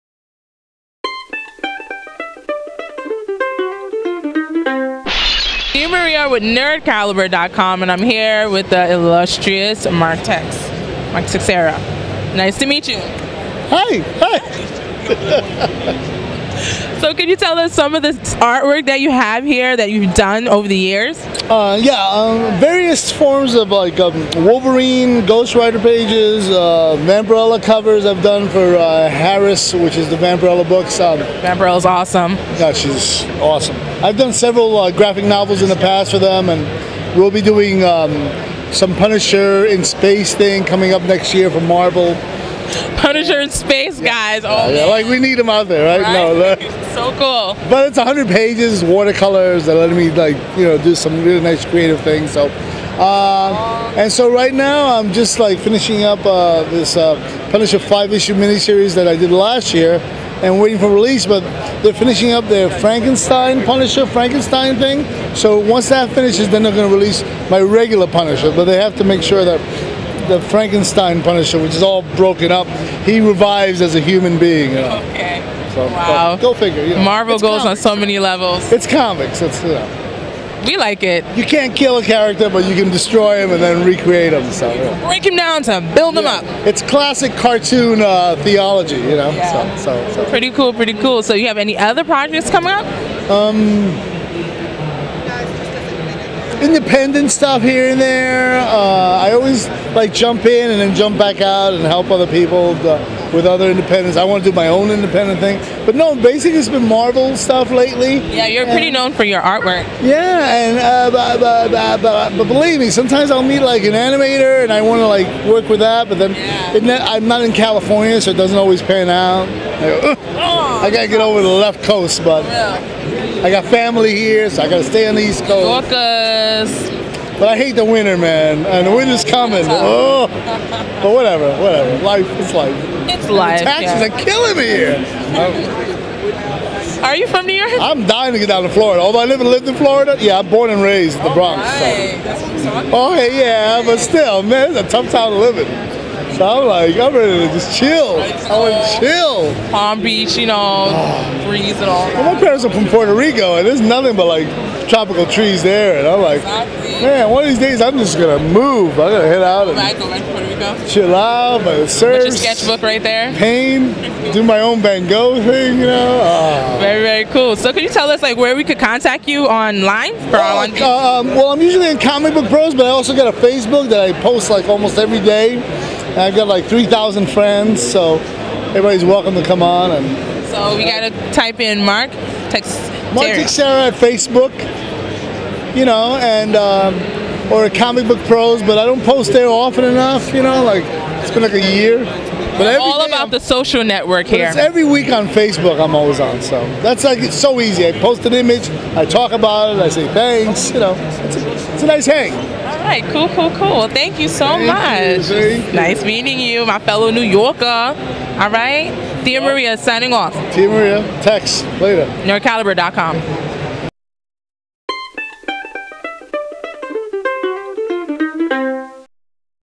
Mark Texeira Interview
Check out our interview with legendary Marvel and DC Comics artist Mark Texeira about his work on the Punisher as well as what he thinks about weather in NYC.

According to Wikipedia: Mark Texeira was born and raised in New York City. He attended Manhattan's High School of Art and Design, and was granted a Presidential Scholarship at the School of Visual Arts, where he attended for two years before dropping out to pursue a freelance commercial art career. During this period, Texeira took classes at the Art Students League. His oil paintings soon won mentions at the Salmagundi Club and the Society of Illustrators, which led to work on book, magazine, and record album covers.[citation needed]

Cover to Sabretooth #2 (September, 1993).

Comics titles Texeira has contributed to include Jonah Hex and its spinoff Hex (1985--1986), Psi-Force (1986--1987), Punisher War Journal (1990), Ghost Rider vol. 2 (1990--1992, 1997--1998), and vol. 5, Wolverine vol. 2 (1993), Sabretooth (1993), Spider-Man: Legacy of Evil (1996), Black Panther vol.3 (1998), Moon Knight vol. 4 (1999), Vampirella, Cyclops (2001), Hercules vol. 4 (2005) and Wolverine vol. 3 (2005).